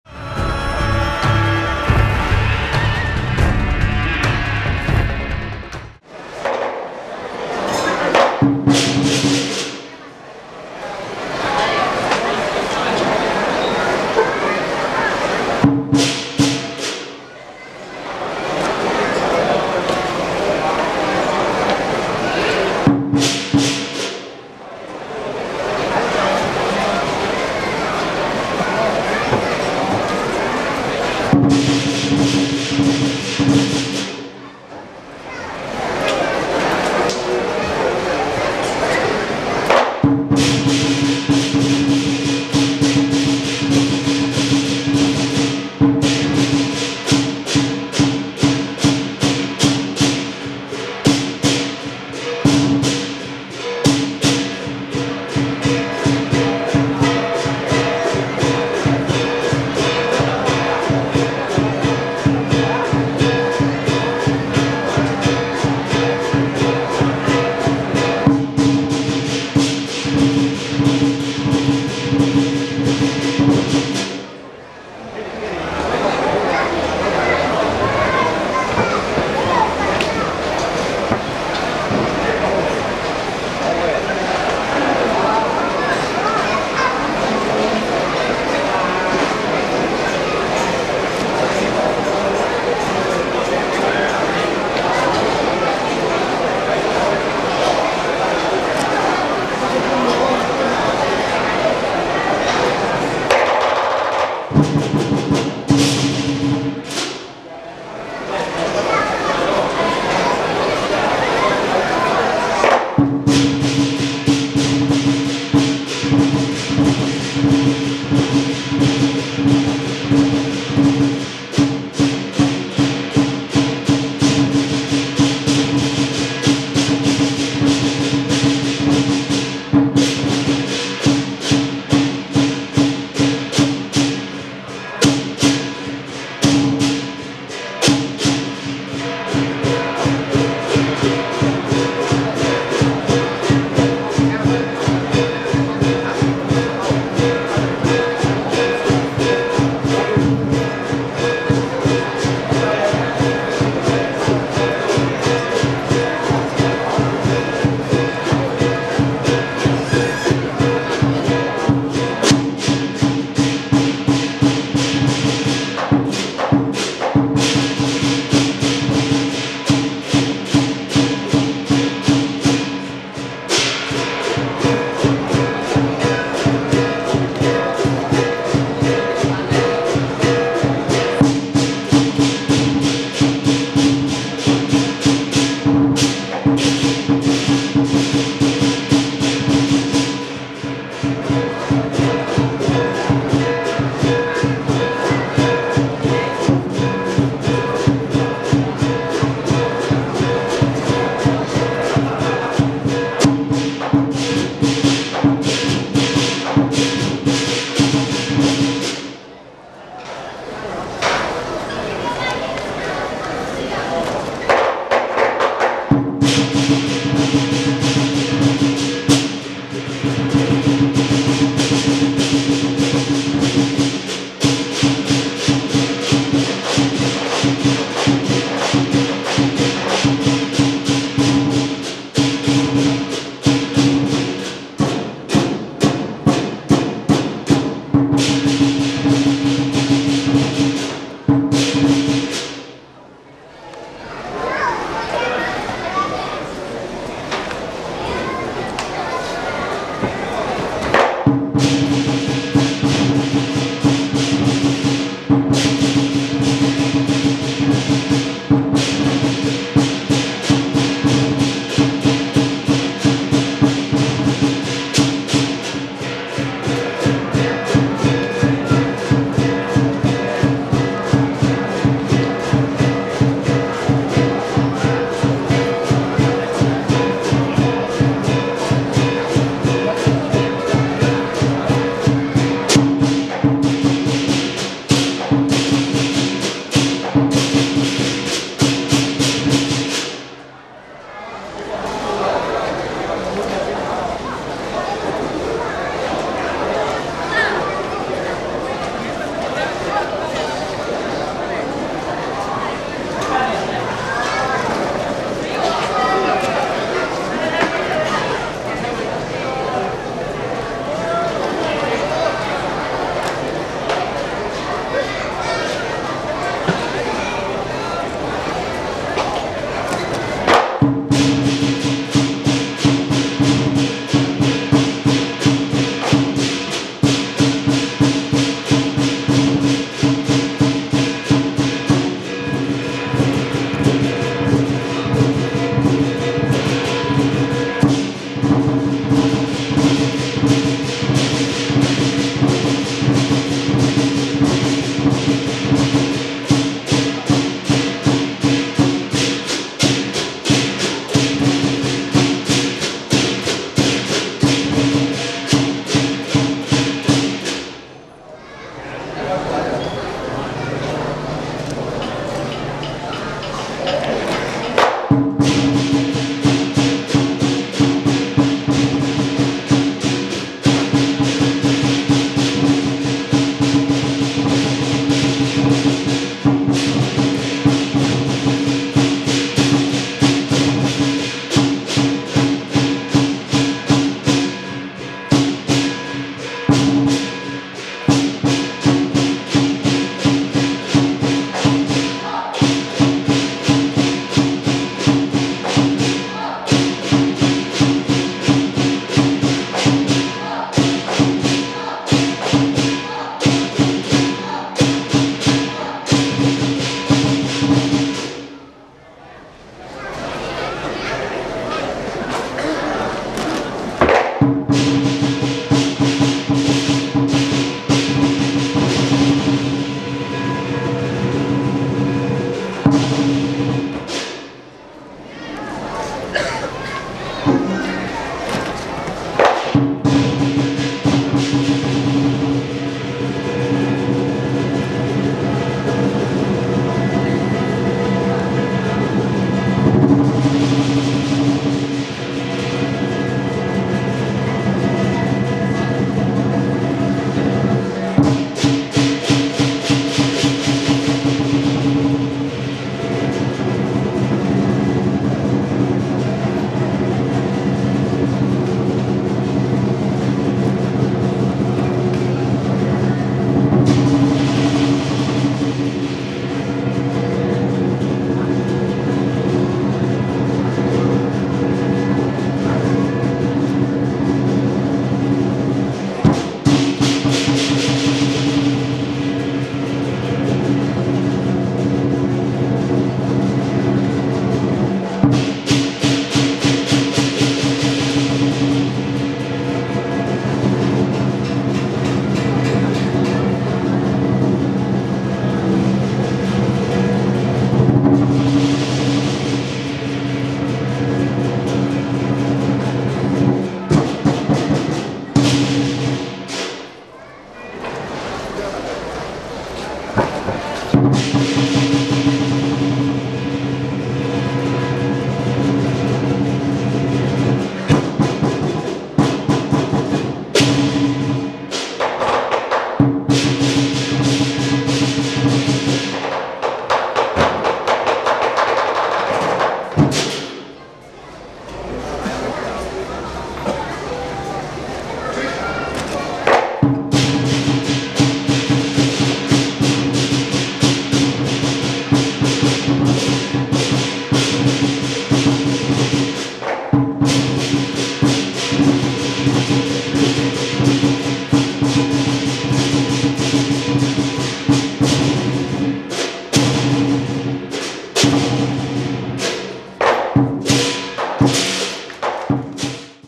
雪兰莪安邦金英龙狮体育会B队
2011年马来西亚安邦第5届南狮传统狮艺赛